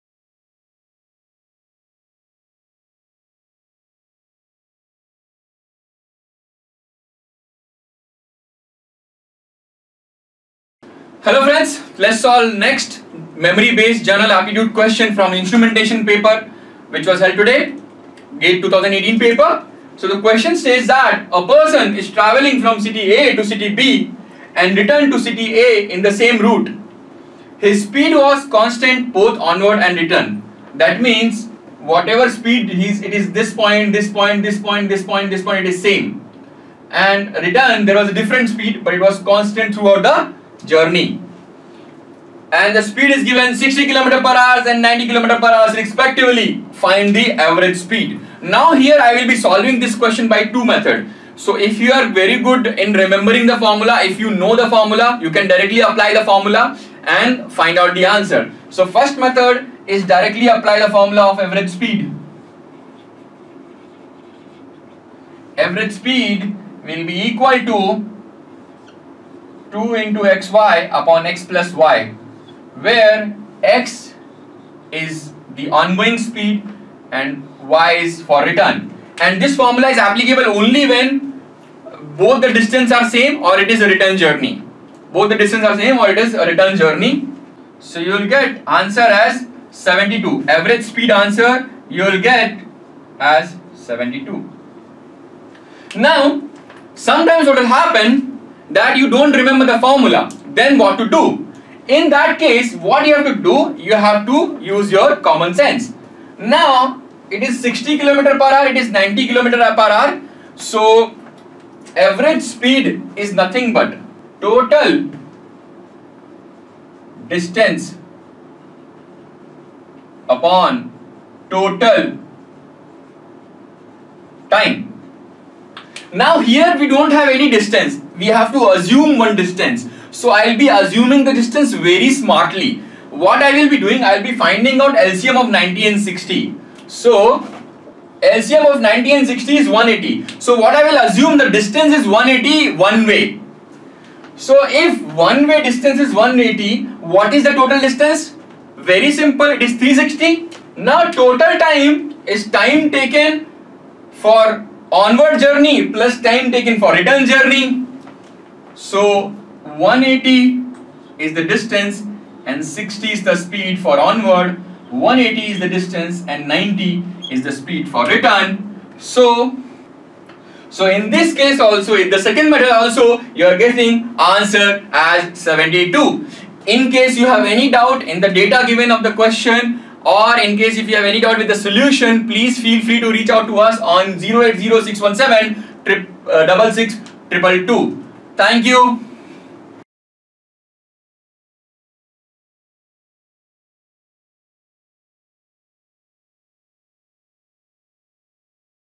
General Aptitude | 04 | Instrumentation Engineering | GATE 2018 Exam Solution
A person is travelling from city A to city B and return to city A in the same route. His speed was constant both onwards and return and the rate was 60 km/hrs and 90 km/hrs respectively. Find the average speed.